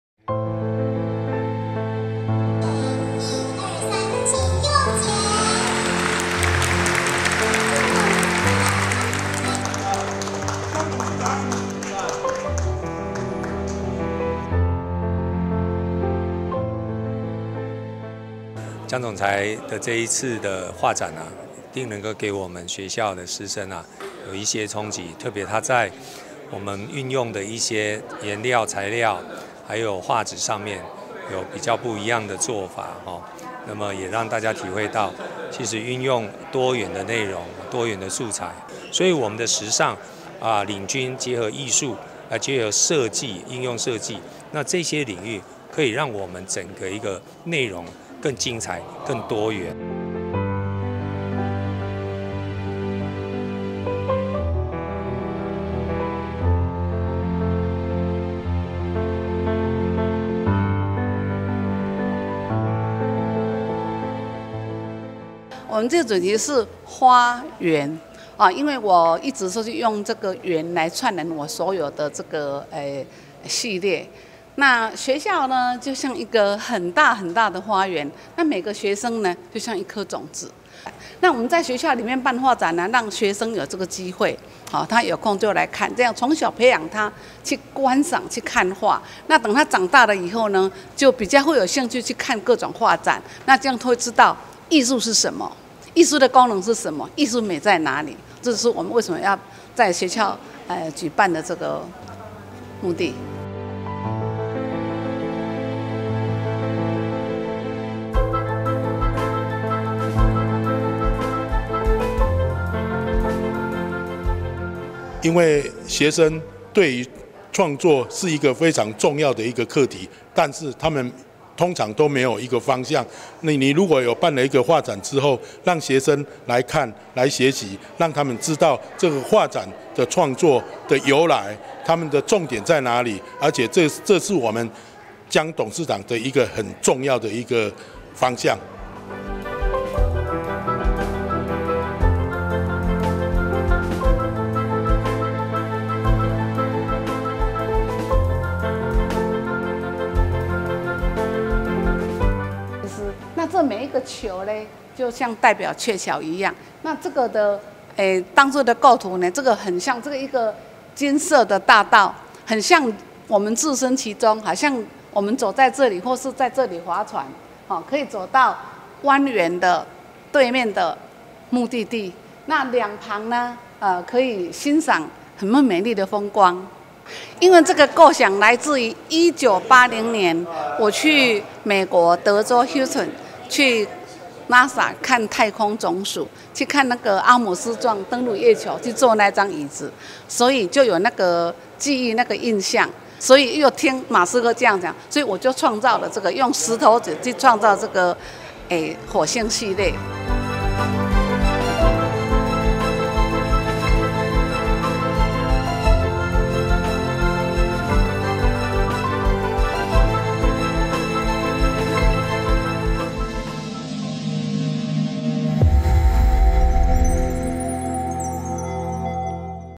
花．圓—江屘菊創作個展 嶺東科技大學藝術中心
嶺東科技大學藝術中心(聖益樓一樓)
臺中市南屯區嶺東路一號
展覽時間｜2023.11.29(三)-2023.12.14(四) 每日10︰00-16︰00
「花．圓」以「圓」象徵能量，隱喻宇宙中所有事物形成的的最小單位元素。創作者江屘菊本次展出「女人花．瓶中語」、「銀河球系列」、「火星探險系列」三個系列，從生命的循環與自然中，尋求萬物事理。展出內容，以「女人花˙瓶中語」比擬生命的成熟與多種樣態，「銀河球系列」展現生命之間的關愛，「火星探險系列」想像未來世界的美好。「圓」貫穿了所有繪畫主題，以不同的生命姿態展現各自的美。

兩個系列圍繞著生命的美好︰「女人花．瓶中語」以綻放的花來暗喻女性對愛的各種表達方式。每一瓶花像是不同姿態的女性，將她們最美的樣子展現在觀賞者眼前。「銀河球系列」構想來自中國神話中「牛郎織女鵲橋相會」的「鵲橋」，天河之上的美麗與浪漫，幻化成創作者的「銀河球」之路，「銀河球」象徵人的意識與思維流動，創作者希冀人與人之間充滿這樣真誠與愛。而「火星探險系列」創作靈感原自早期到NASA的旅行經驗，以及2022年Tesla執行長暨SpaceX執行長馬斯克（Elon Musk）致力於火星移民計畫的新聞。火星古稱熒惑，因其熒熒如火，位置、亮度時常變動讓人無法捉摸而稱之為熒惑，是離太陽第四近的行星，而我透過科學家的研究，了解火星的各種樣貌，以宏觀、微觀的方式表現我對火星的想像。

主辦單位「嶺東科技大學藝術中心」坐落校園一隅，典雅幽靜，創作者欲以畫作展現生命之愛，以「花」展現繁花盛開、萬物美好，以「銀河」隱喻此精神的淵源流長，以「火星」描摹美麗的未來。嶺東科技大學創辦人與董事長為懷念故鄉「嶺東」，並以此命名，因而規劃展出「女人花．瓶中語系列」、「銀河球系列」、「火星探險系列」，呼應學校創辦宗旨，恰如其分地展現生命之美。希冀本展「生命的圓滿」在作品中，在你、我之間，在生活的每一刻。